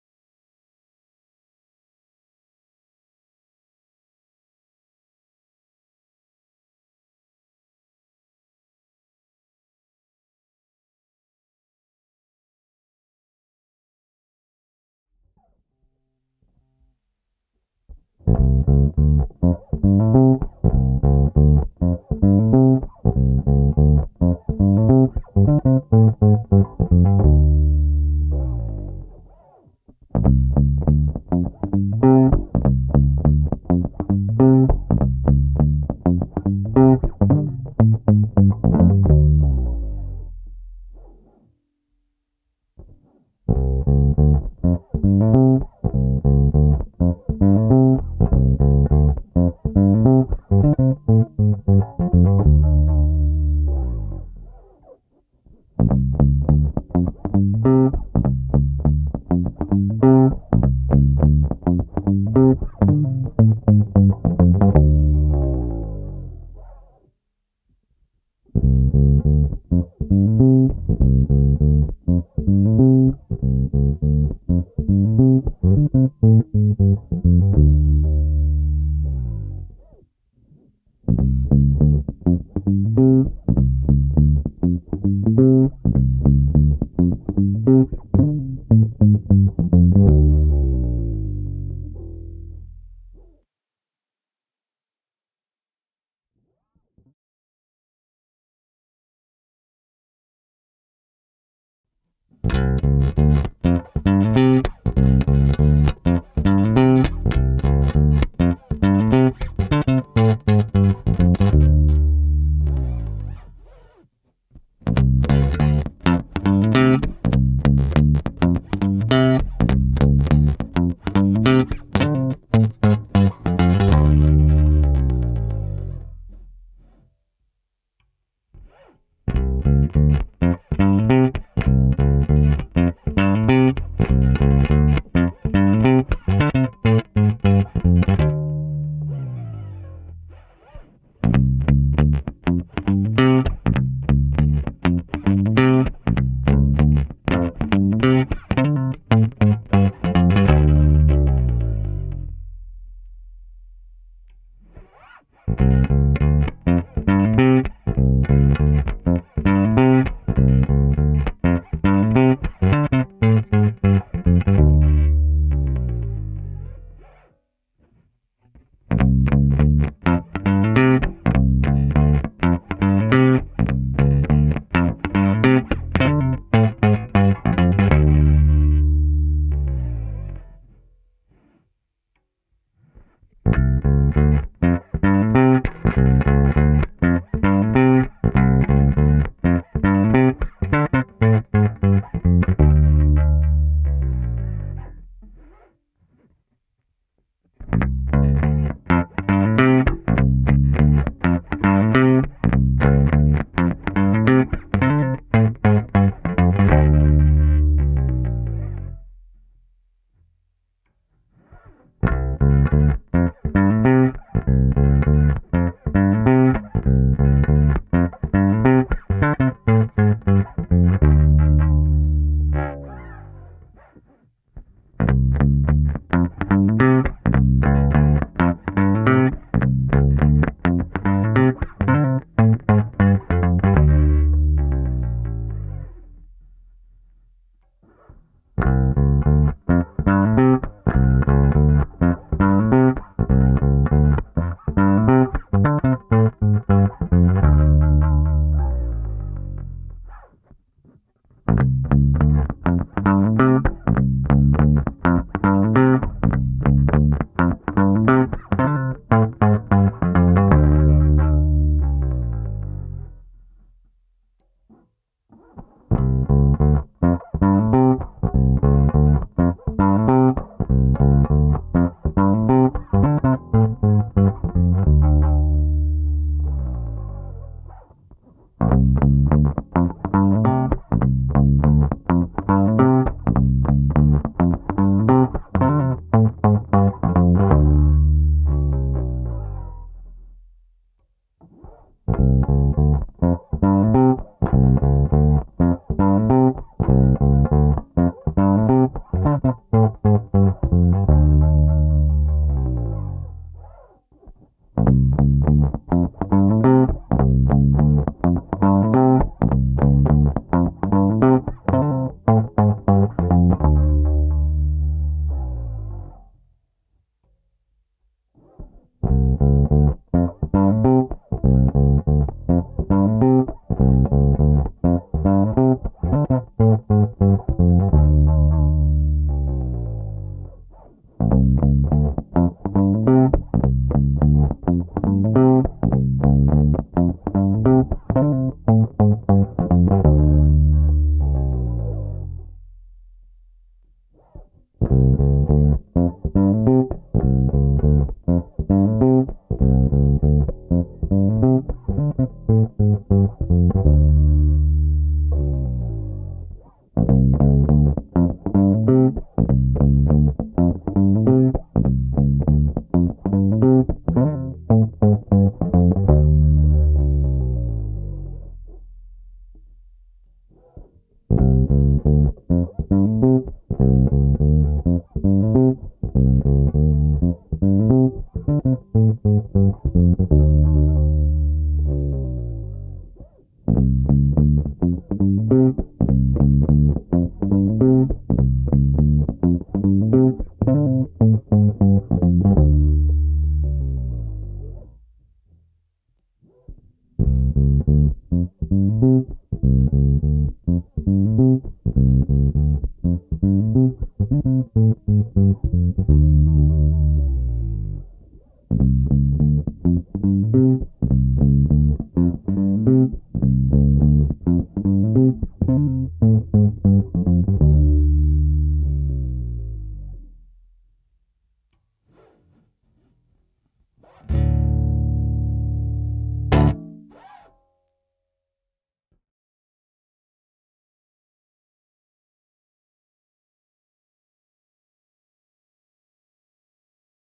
BQmusic V12B bass tone control demo
Demo by Juande Music of the BQmusic V12B 12 step tone control installed in a Fender Musicmaster.
The V12 tone control is also available for guitar (V12G) and can be custom made in simpler 9 and 7 steps versions.